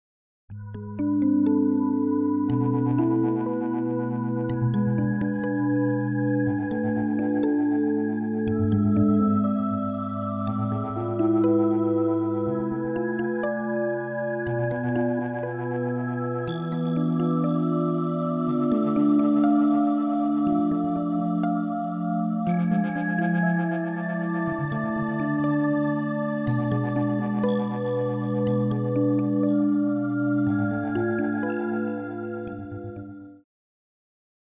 Art, Ally Pally and the RBA
Second introduction to the RBA exhibition (July 4-14) and Ally Pally art presentations (July 8) at Mall Galleries